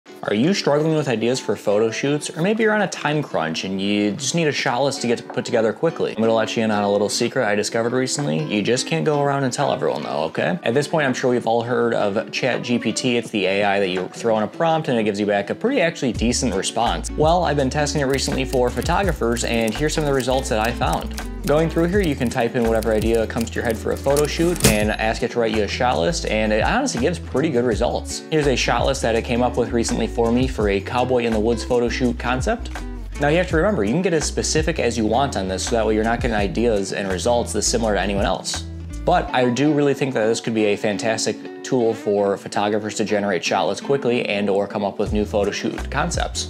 Using AI for Photography📷
In this video, I show you how to use AI to better improve your photography!